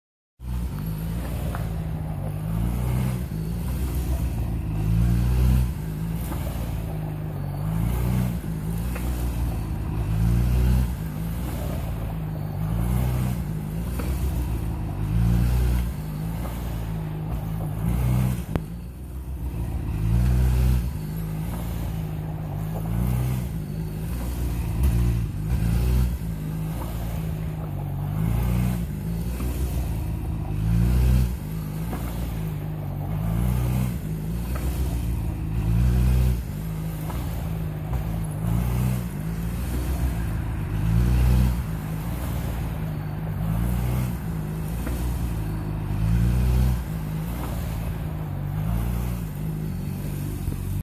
Ni siquiera Jorge Lorenzo Realiza tan bien el Ocho Corto sobre una 600.
Xavier Zayat de Nueva York, posee el Record Mundial de ochos cortos, sobre una 600cc y lo ha conseguido en la puerta de su casa, impresionante. Y todo esto a traves de nuestros Cursos Online.